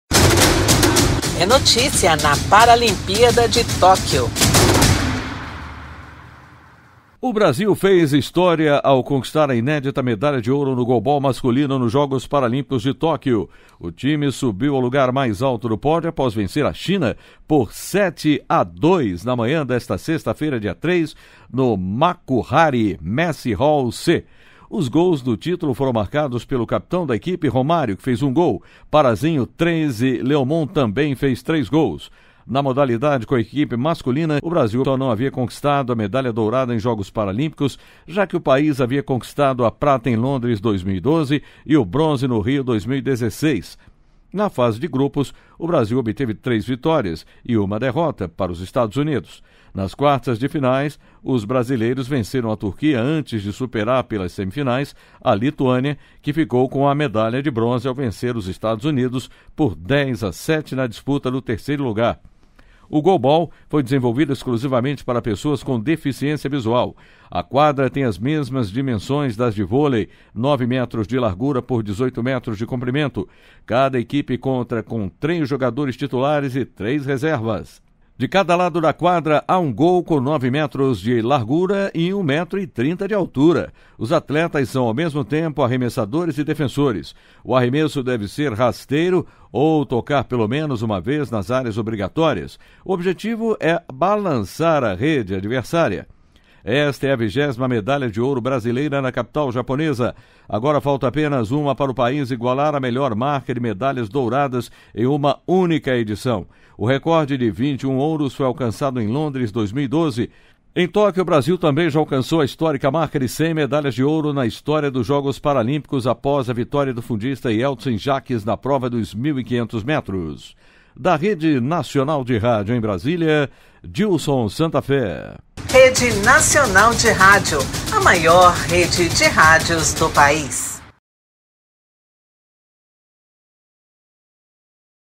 03/09/2021 - PARALIMPÍADA DE TÓQUIO - BRASIL É OURO NO GOALBALL MASCULINO
O time subiu ao lugar mais alto do pódio após vencer a China por 7 a 2, na manhã desta sexta-feira (3).

O Brasil fez história ao conquistar a inédita medalha de ouro no goalball masculino nos Jogos Paralímpicos de Tóquio. O time subiu ao lugar mais alto do pódio após vencer a China por 7 a 2, na manhã desta sexta-feira, 3, no Makuhari Messe Hall C. Os gols do título foram marcados pelo capitão da equipe, Romário (1), Parazinho (3) e Leomon (3).

Na modalidade, com a equipe masculina, o Brasil só não havia conquistado a medalha dourada em Jogos Paralímpicos, já que o país havia conquistado a prata em Londres 2012 e o bronze no Rio 2016.

Na fase de grupos, o Brasil obteve três vitórias e uma derrota, para os Estados Unidos. Nas quartas de finais, os brasileiros venceram a Turquia antes de superar pelas semifinais a Lituânia, que ficou com a medalha de bronze ao vencer os Estados Unidos por 10 a 7 na disputa do terceiro lugar.

o goalball foi desenvolvido exclusivamente para pessoas com deficiência visual. A quadra tem as mesmas dimensões das de vôlei (9m de largura por 18m de comprimento). Cada equipe conta com três jogadores titulares e três reservas. De cada lado da quadra, há um gol com 9m de largura e 1,30m de altura. Os atletas são, ao mesmo tempo, arremessadores e defensores. O arremesso deve ser rasteiro ou tocar pelo menos uma vez nas áreas obrigatórias. O objetivo é balançar a rede adversária.

Esta é a 20ª medalha de ouro brasileira na capital japonesa. Agora, falta apenas uma para o país igualar a melhor marca de medalhas douradas em uma única edição. O recorde de 21 ouros foi alcançado em Londres 2012. Em Tóquio, o Brasil também já alcançou a histórica marca de 100 medalhas de ouro na história dos Jogos Paralímpicos, após a vitória do fundista Yeltsin Jacques na prova dos 1.500m.